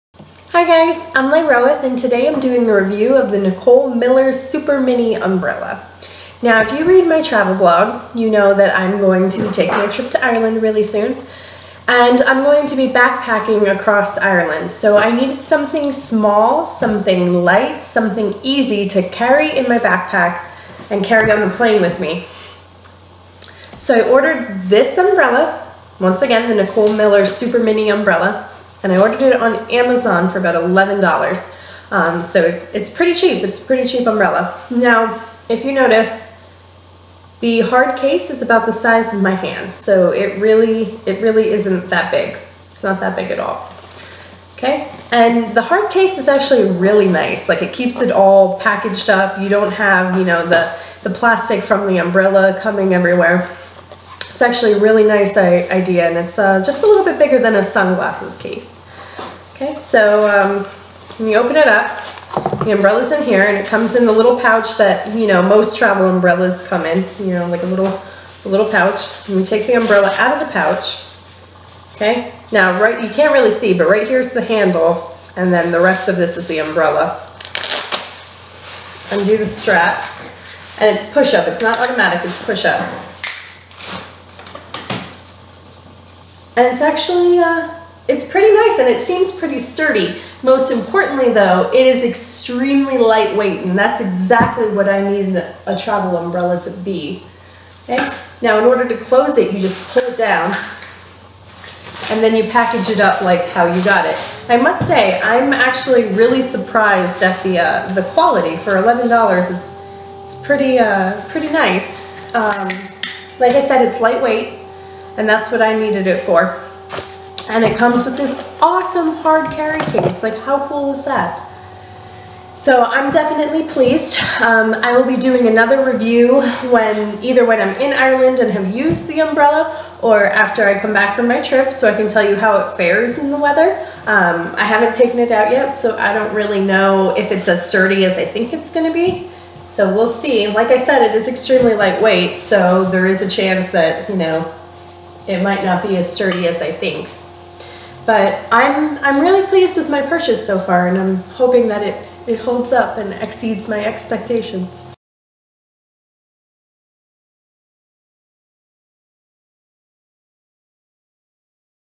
Nicole Miller Supermini Umbrella Review
A review of first impressions of the Nicole Miller Supermini Umbrella.

Link to buy